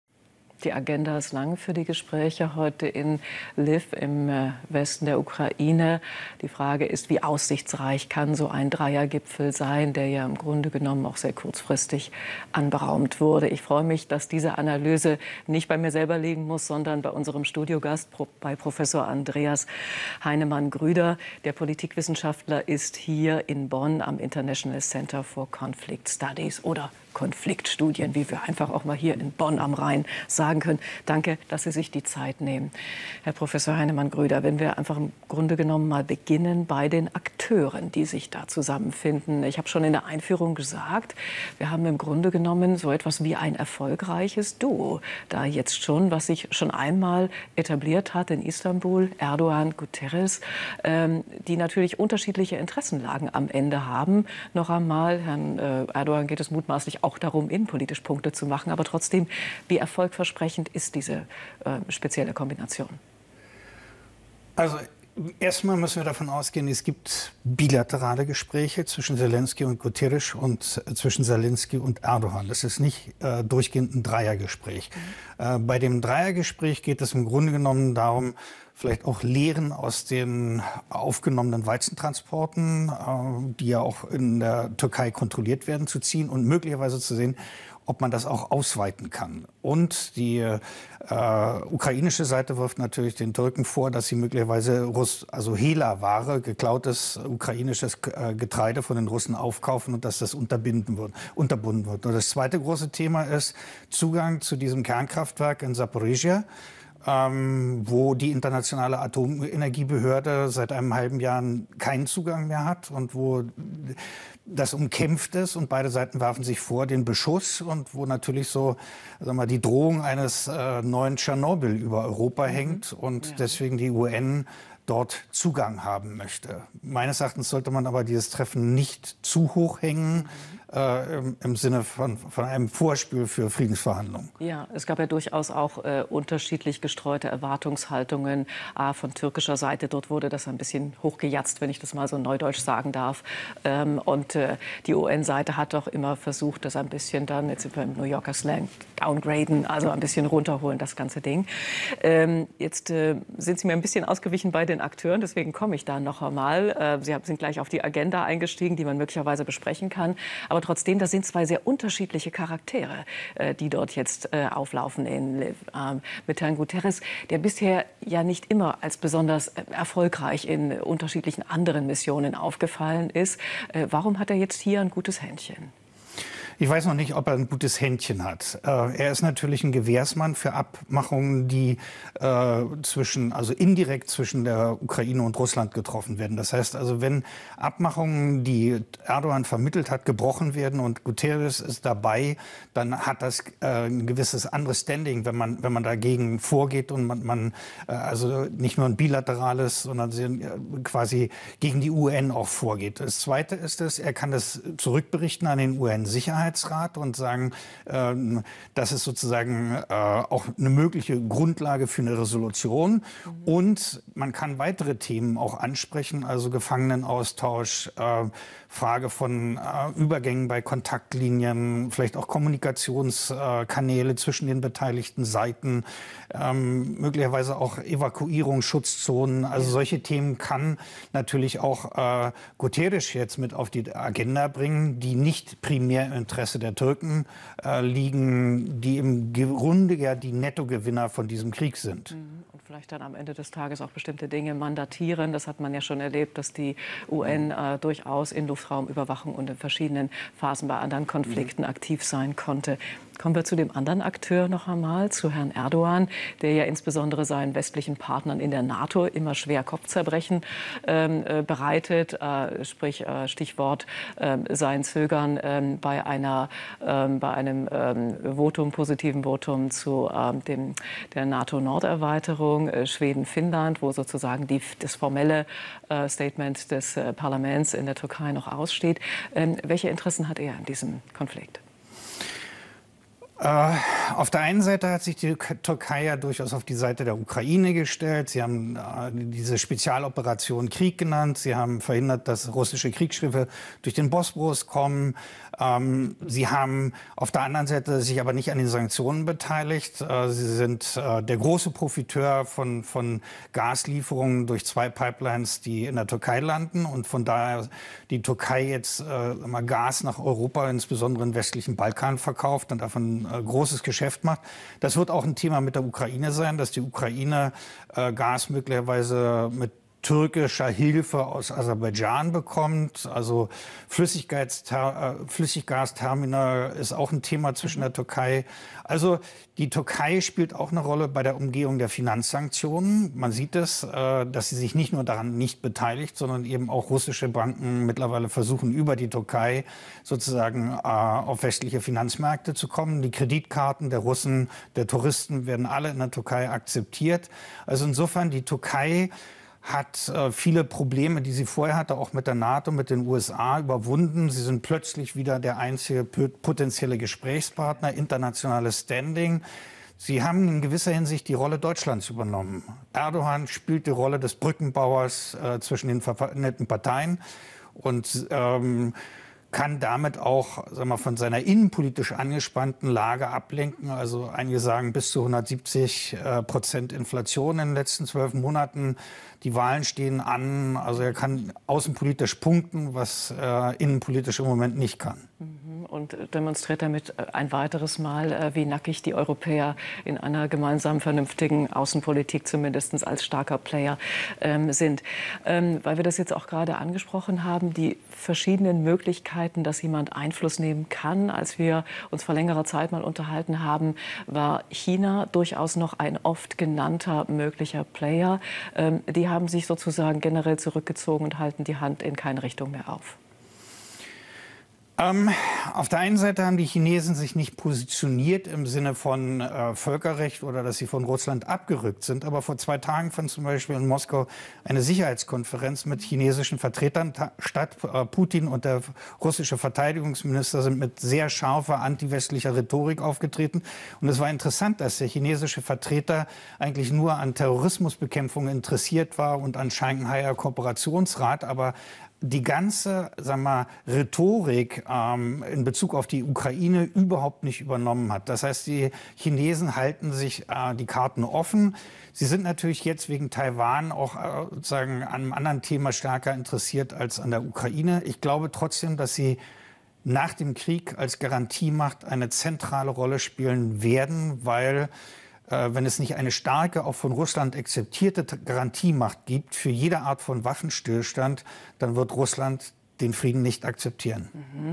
Prof. Andreas Heinemann-Grüder zum Ukraine-Krieg am 18.08.22
Prof. Andreas Heinemann-Grüder, Bonn International Centre for Conflict Studies, zum Ukraine-Krieg.